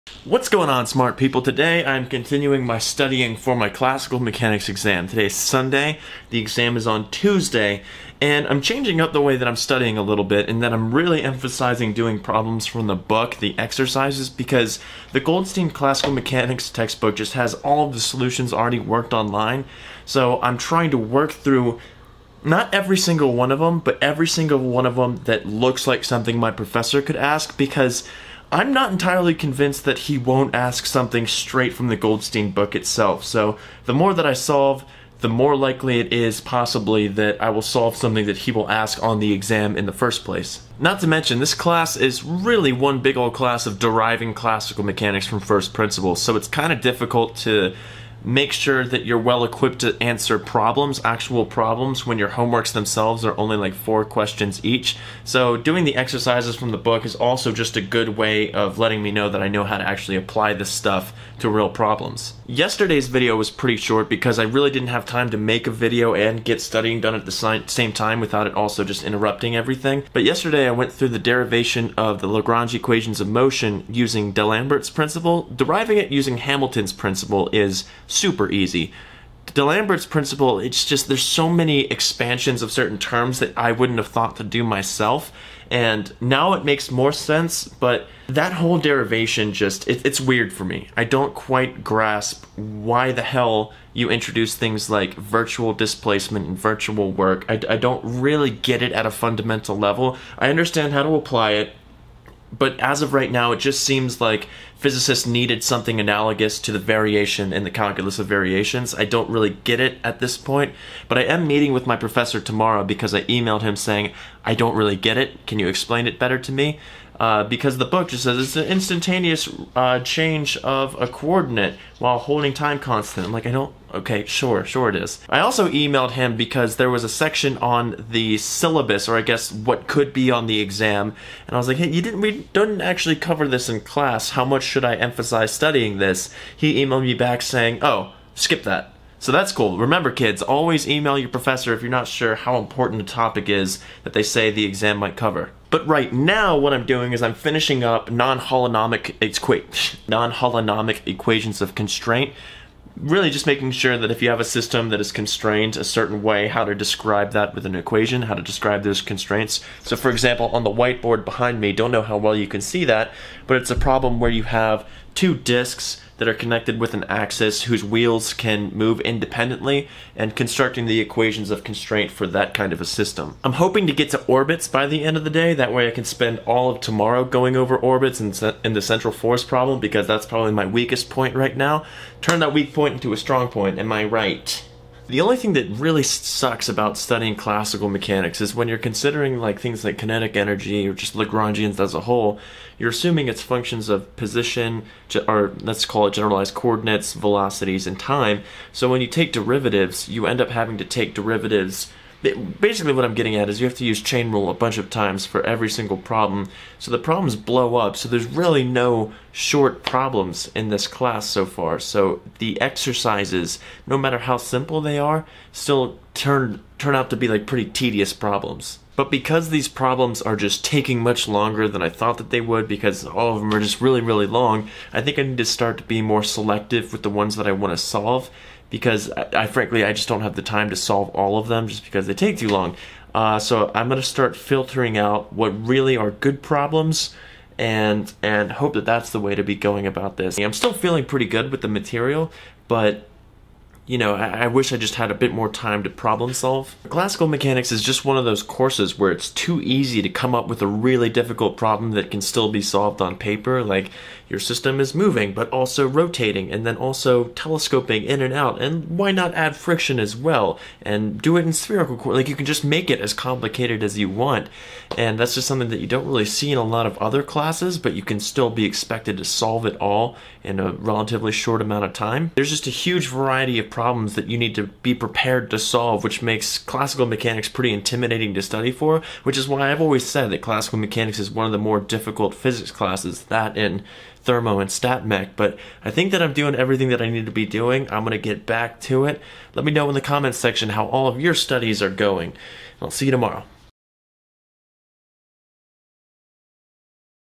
Studying For Classical Mechanics pt. 2
My classical mechanics  midterm for gradschool is in two days! Today I knock out a good amount of studying for it and discuss what's left.